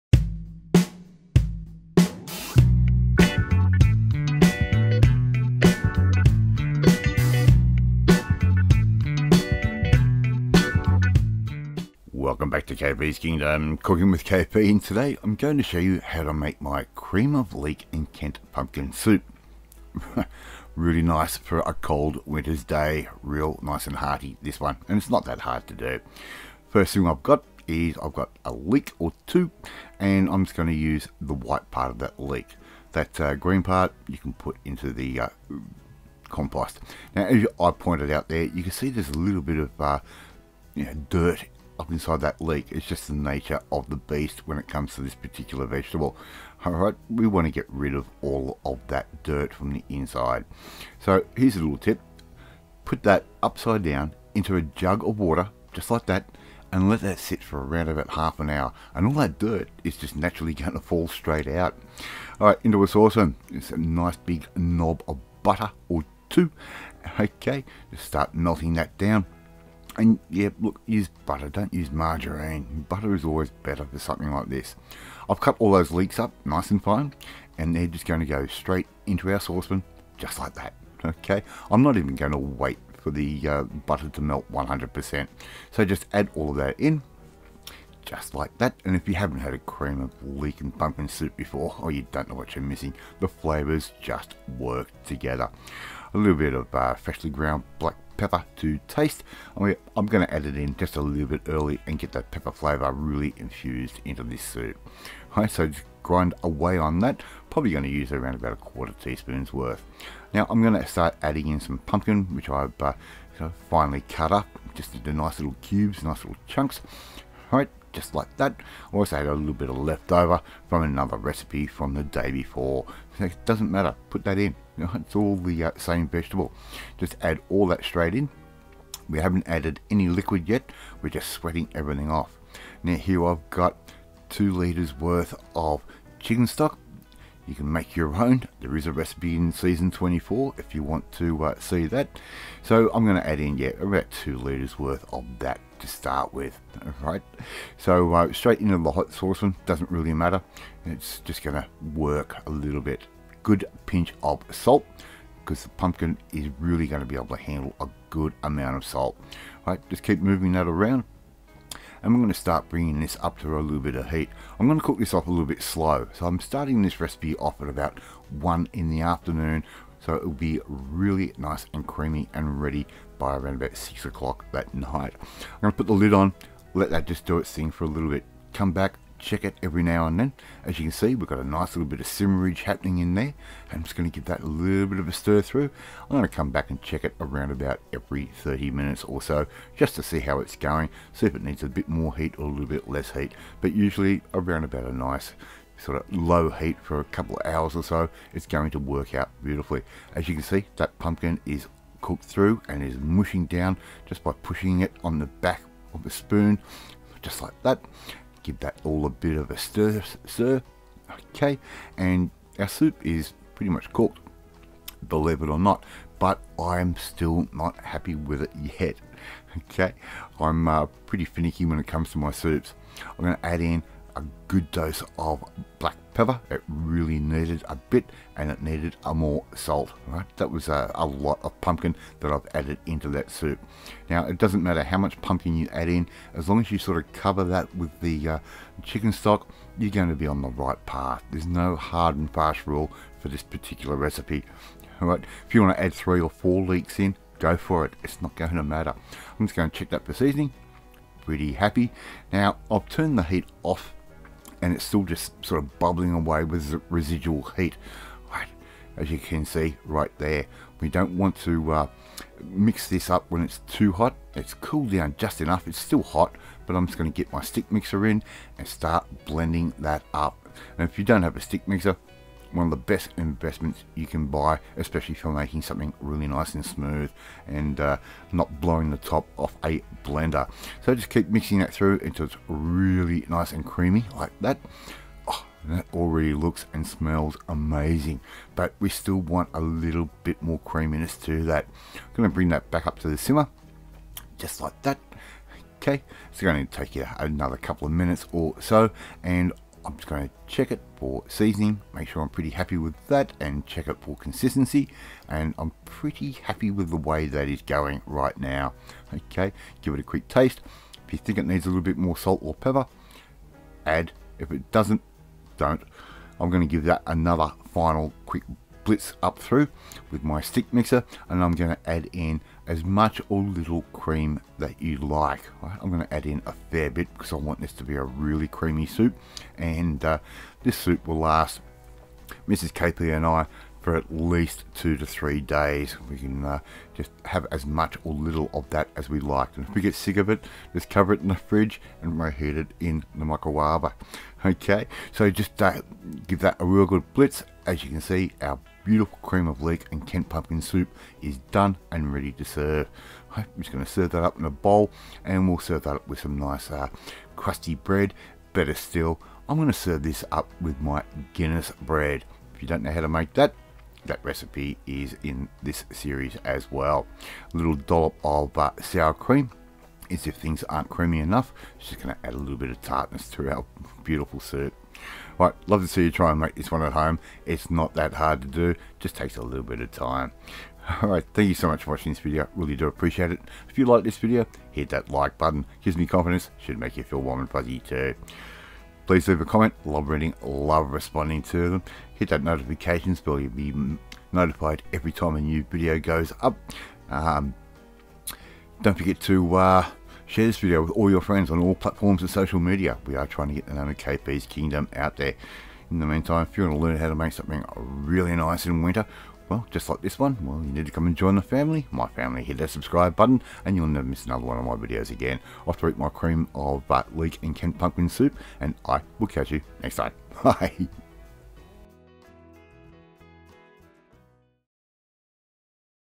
Cream of Leek and Pumpkin Soup - Cook with K.P SE 25 EP01
Watch and learn how to make my cream of leek and pumpkin soup, this creamy and hearty soup is very inexpensive to make and is perfect for those real cold days when nothing will warm you up.